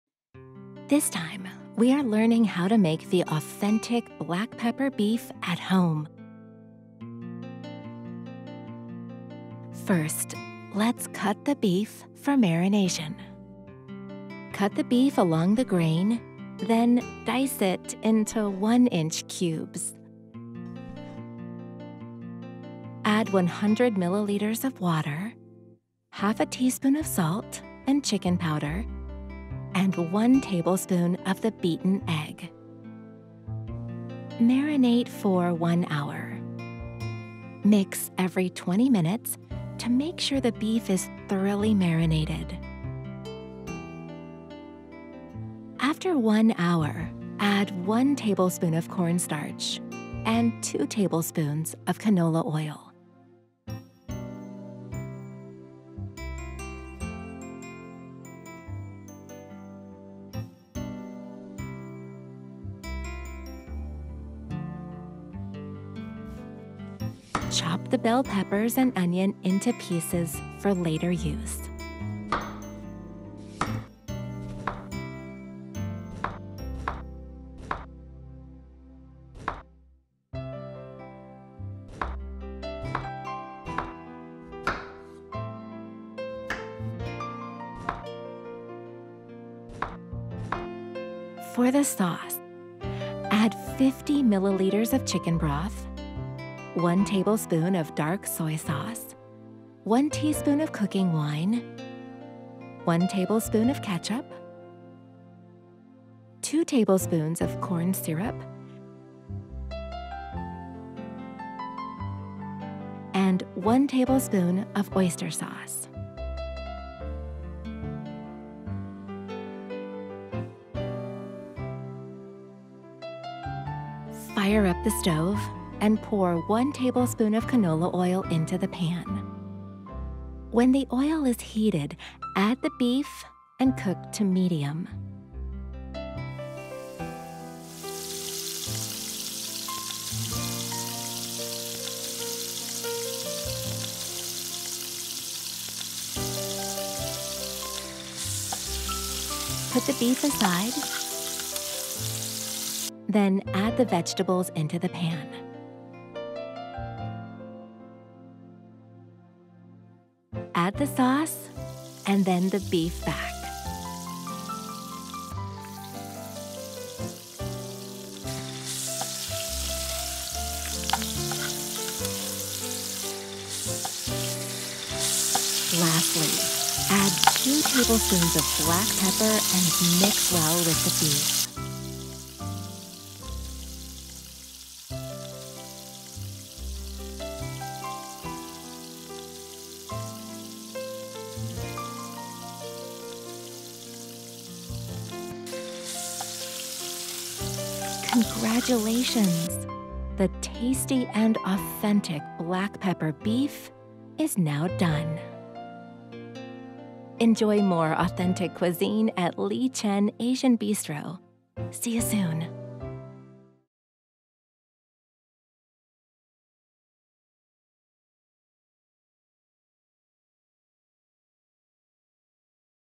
How to Make Authentic Black Pepper Beef at Home with Lee Chen Asian Bistro
How to make our signature Black Pepper Beef!

Scallion Noodle Ingredients:
• Flank Steak 400 g
• Chicken Powder 1/2 teaspoon
• Beaten Egg 1 tablespoon
• Corn Starch 1 tablespoon
• Canola Oil 2 tablespoons
• Bell Pepper 1/2
• Onion 1/2
• Chicken Broth 50 ml
• Dark Soy Sauce 1 tablespoon
• Cooking Wine 1 teaspoon
• Ketchup 1 tablespoon
• Corn Syrup 2 tablespoons
• Oyster Sauce 1 tablespoon
• Black Pepper 2 tablespoons